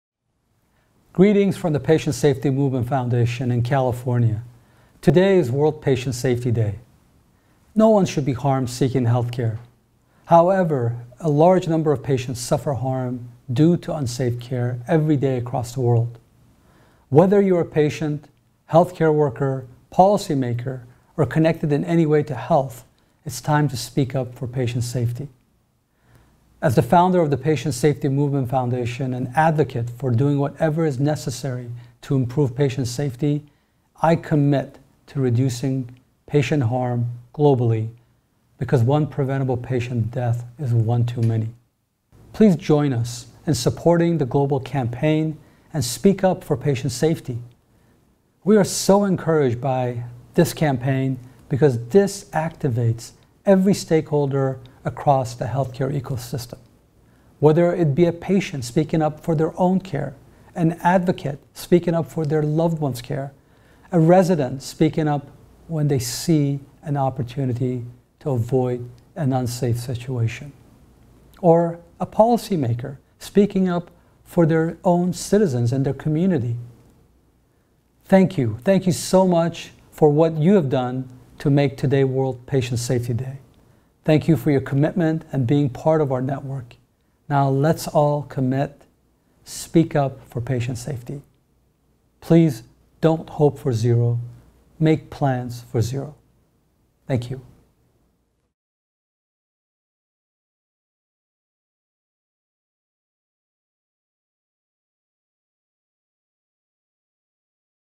Happy World Patient Safety Day from Joe
Our Founder & Chairman Joe Kiani shares the Patient Safety Movement's plans for the 1st Annual World Health Organization (WHO) recognized World Patient Safety Day on September 17, 2019.